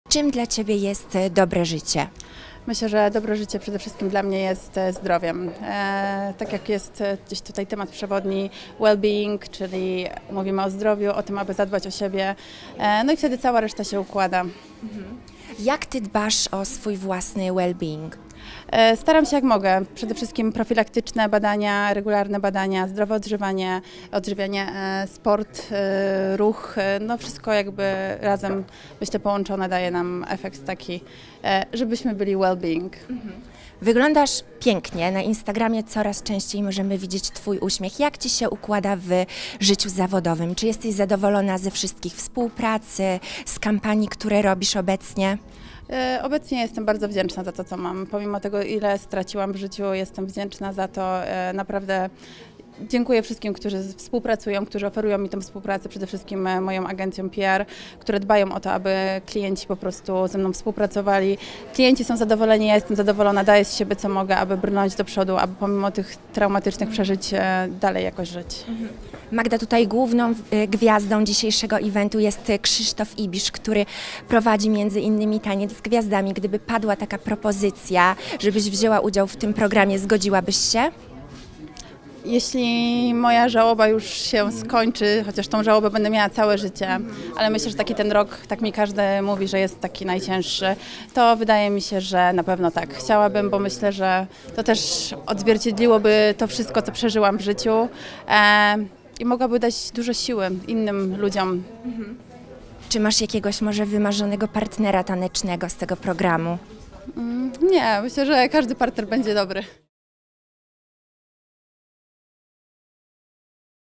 Magdalena Stępień w "TAŃCU Z GWIAZDAMI"?
Magdalena Stępień nie ukrywa, że jej obecność w show biznesie podyktowana jest przede wszystkim realiami ekonomicznymi. Po cichu marzy o "Tańcu z Gwiazdami"... Uda się jej spełnić to pragnienie?